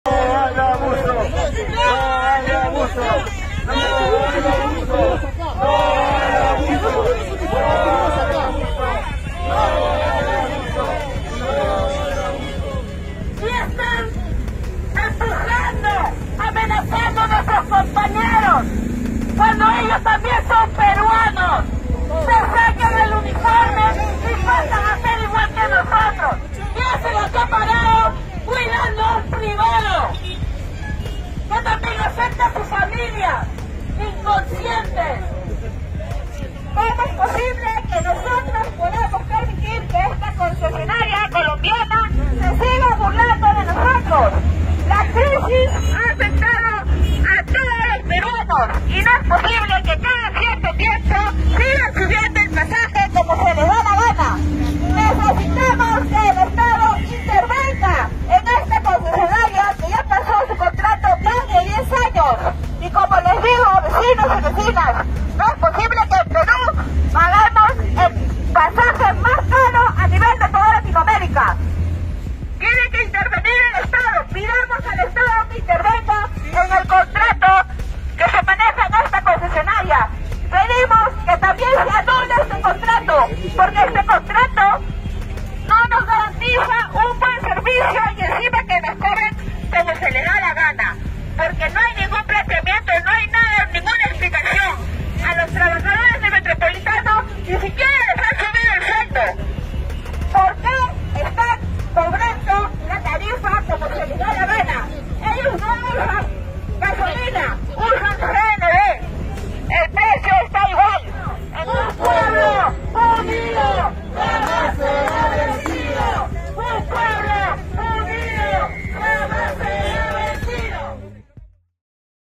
Plantón en estación de Metropolitano por alza de pasajes
Ciudadanos de Lima Norte acudieron a la estación Naranjal para realizar un plantón en contra del aumento de pasajes del Metropolitano.